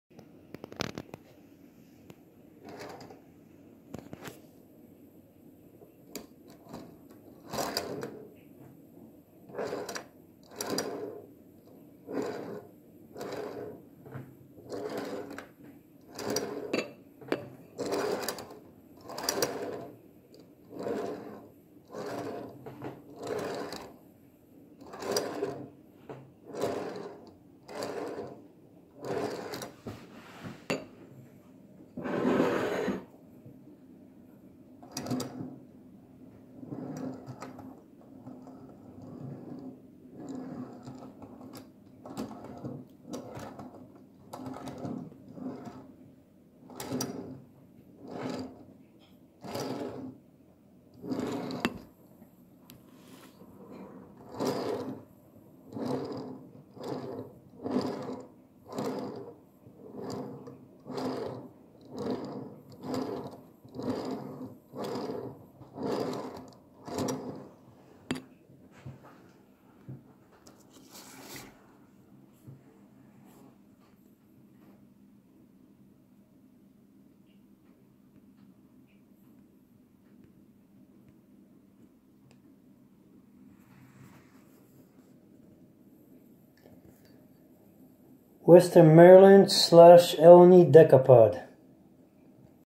Western Maryland, L&NE decapod project in H0 gauge, 1:87 scale.
These huge decapods were build in 1920s by Baldwin Locomotive Works for only two railroads; 20 for Western Maryland and four almost identical ones for Lehigh and New England. The ones for L&NE had booster engines on the tenders, making them the most powerfull decapods in the world. None were saved. Wanting to have a model of one i decided to scratch build one. No manufacturer has made them in large numbers and brass ones are not only hard to find, but also extremely expensive. It took several months and many trips to different hobby shops and train shows to find plans, data and several good enough pictures to work out the details on these machines. This projest has been going on for few months when i have spare time and today i started on side rods which is the most critical part for model to work properly. I figured enough is built now to share it here, enjoy. More is coming.